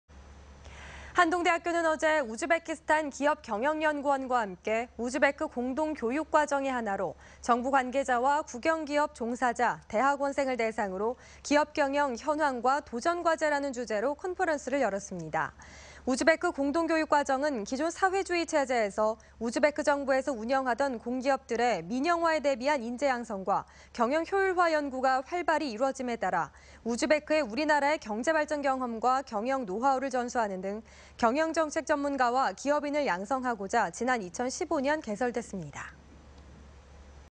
[포항MBC뉴스]한동대, 우즈베크와 기업경영 국제콘퍼런스 개최
한동대, 우즈베크와 기업경영 국제콘퍼런스 개최